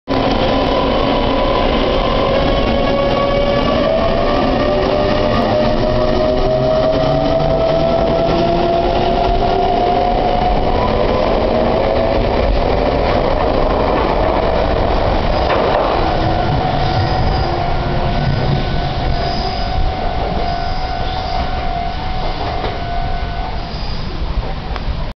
名鉄日本ライン今渡駅 3500系発車
普通 新可児ゆき (日本ライン今渡 17：06発)